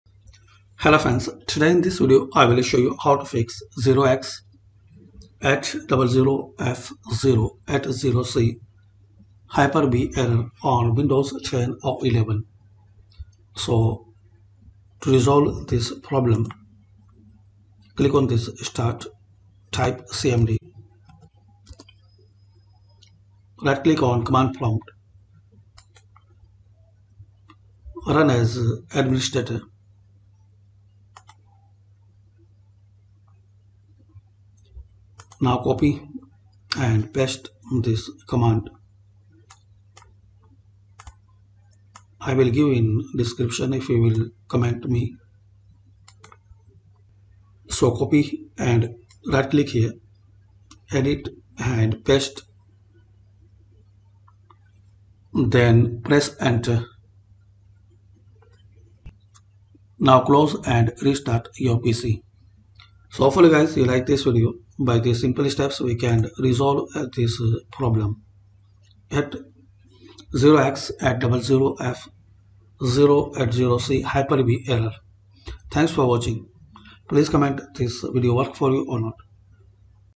Fix 0x800f080c Hyper V Error on Windows 11
This Tutorial Helps to Fix 0x800f080c Hyper V Error on Windows 11

00:00 Intro
00:11 Run DISM and SFC Scans.
01:09 Closing

Command Used:
Enable-WindowsOptionalFeature -Online -FeatureName Microsoft-Hyper-V-All